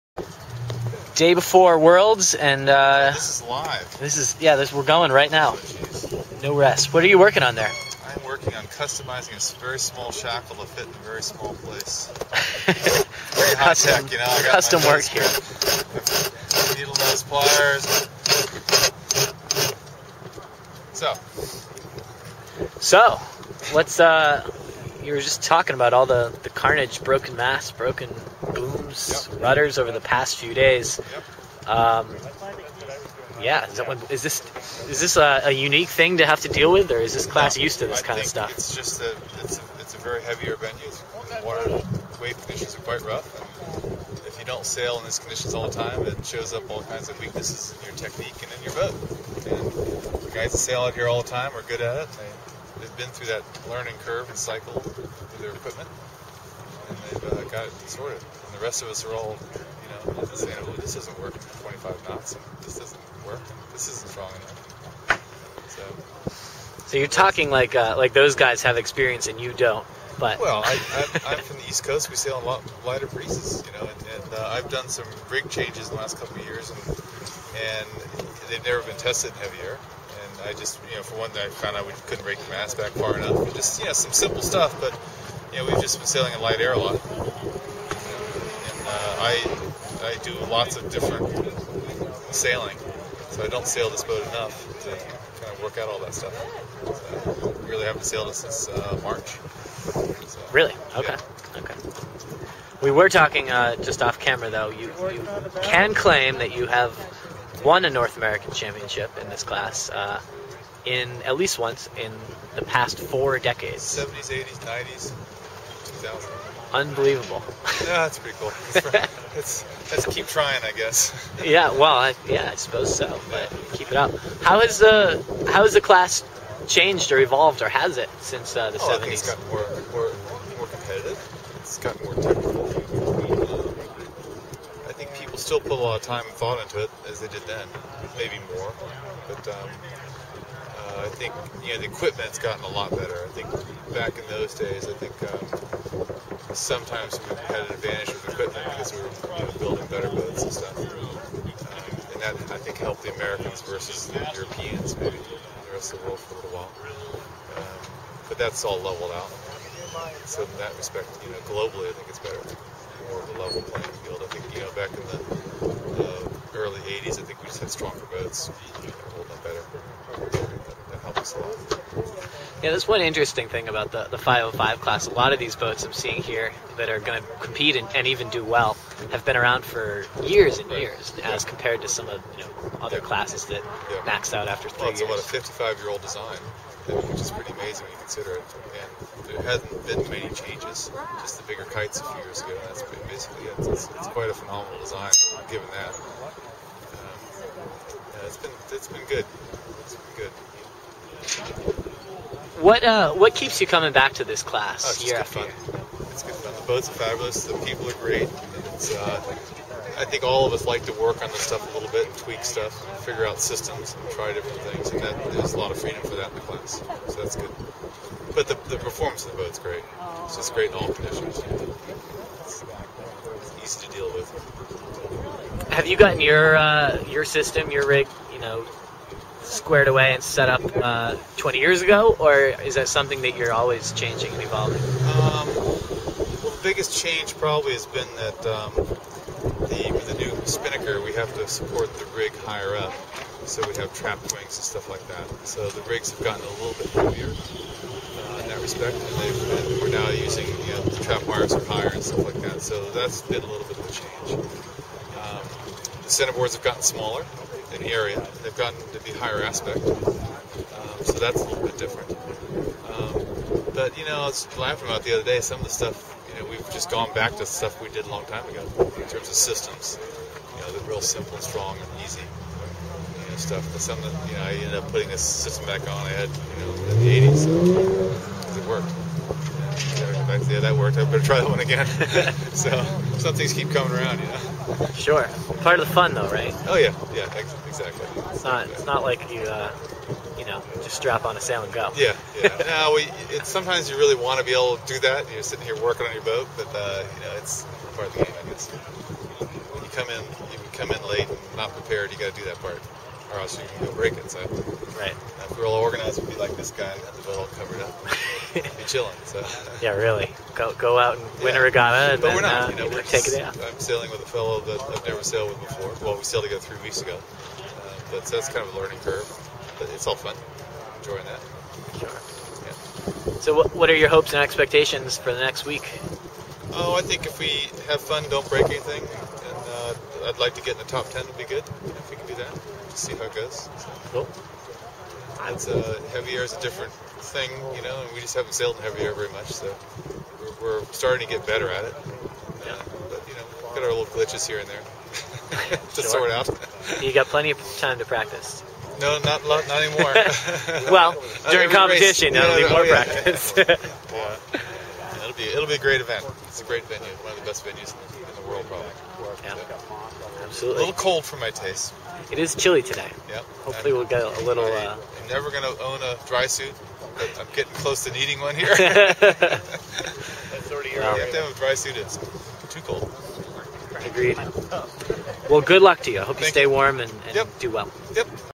Ethan Bixby On The Last Four Decades, SAP 505 World Championship 2009 on Sailgroove
Interview by Sailgroove reporter, San Francisco.